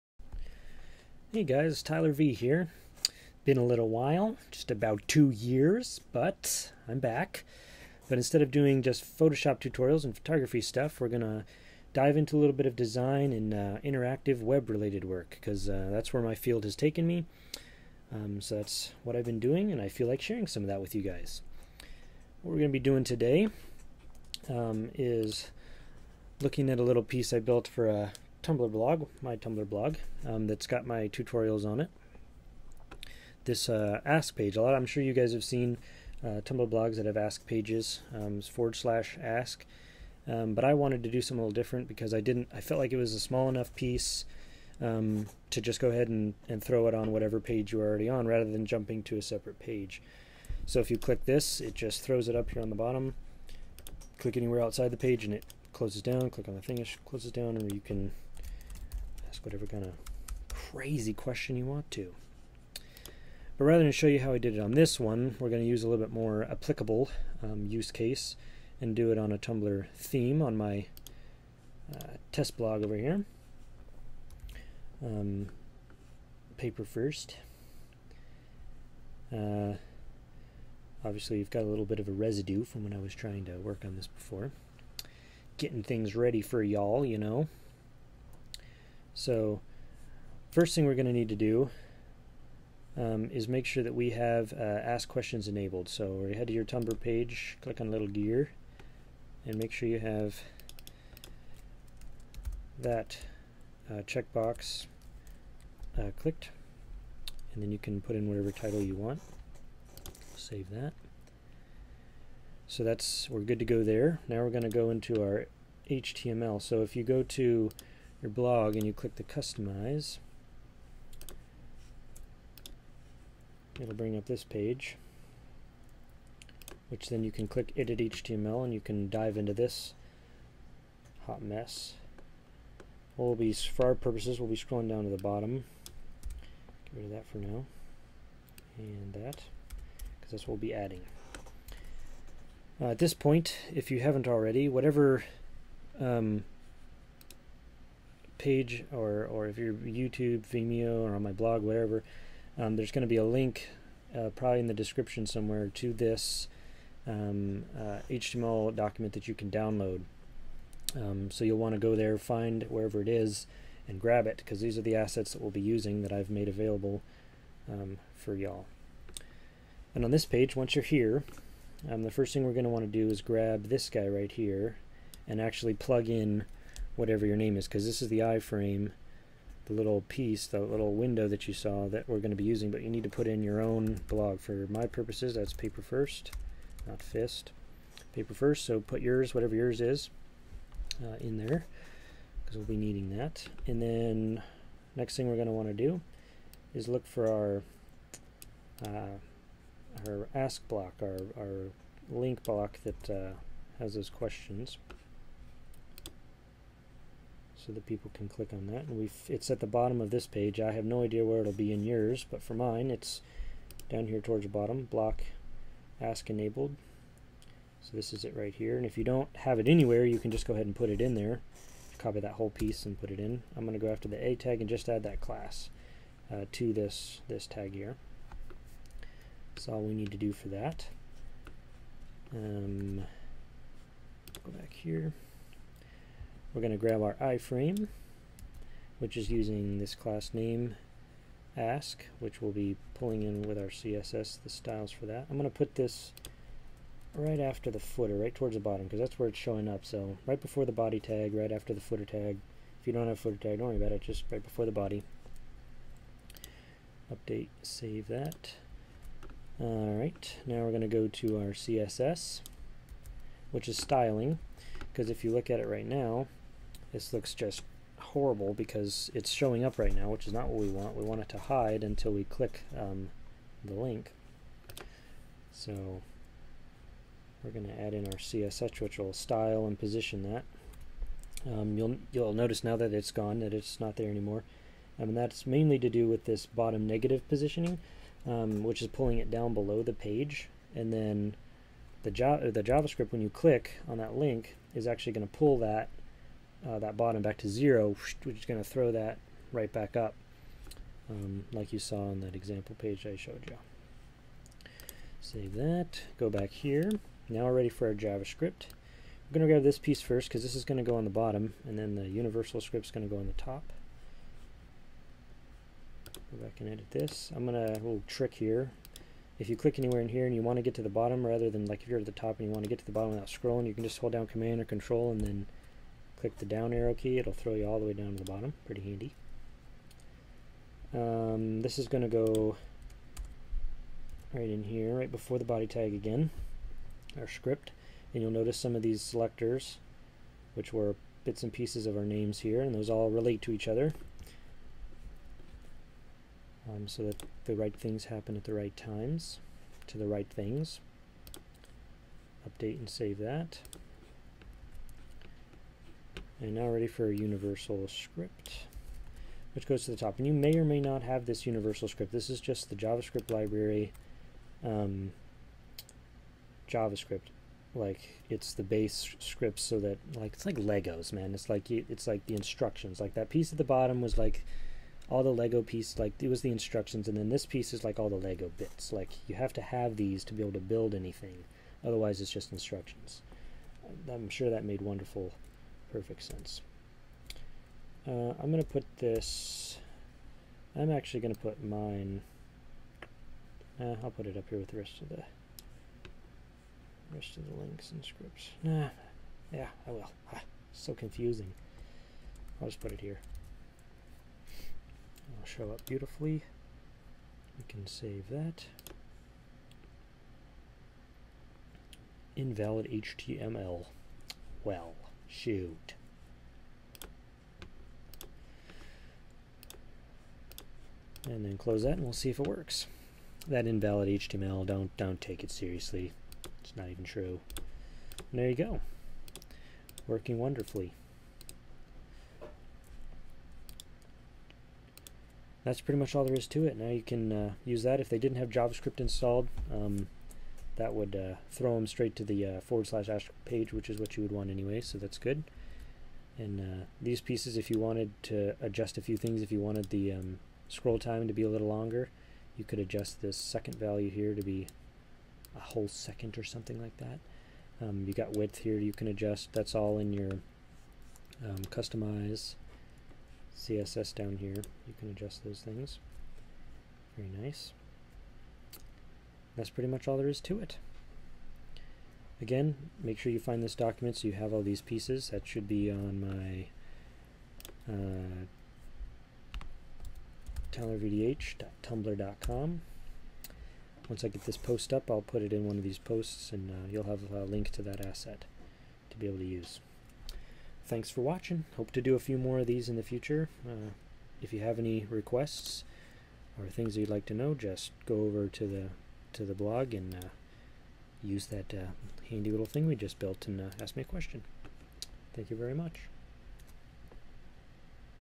Customize Your Tumblr Ask Panel
In this video I'll be covering the specifics of how to turn your Tumblr blog ask page into an iframe that can simply pop up and down from any page in your blog.